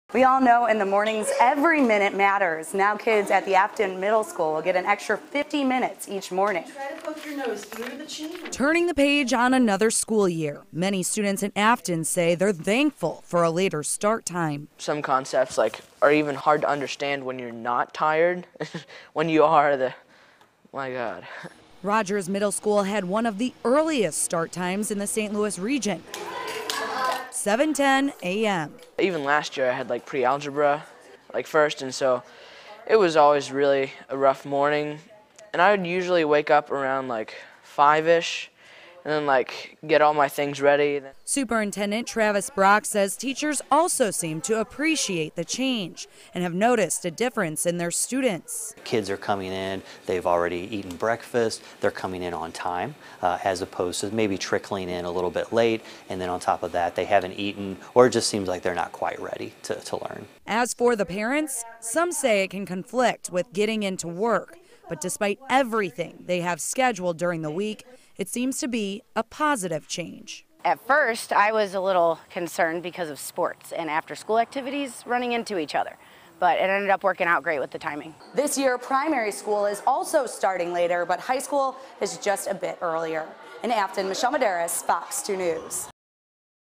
New Start For The School Day In Affton School District
A new school year means a new start time for students in the Affton School District.Fox 2/News 11`s Michelle Madaras visited their middle school to see how a later start is working for students and parents.